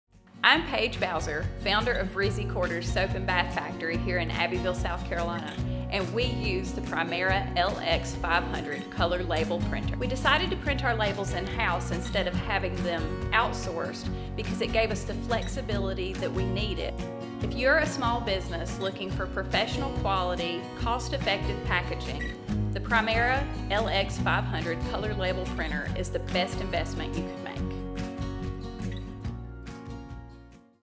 Primera LX500 Color Label Printer Case Study - Soap Factory Makes Their Own Labels - Short Version
Primera Technology visits Paige Bowser, founder of Breezy Quarters Soap and Bath Factory in Abbeville, South Carolina, to find out how their Primera LX500 Color Label Printer is helping their business grow. The LX500 allows them to be creative and gives them the flexibility to design and print a label immediately rather than having to wait with outsourcing. They also appreciate not having to invest in label stock that they wouldn't use. They can print just want they need, when they need it.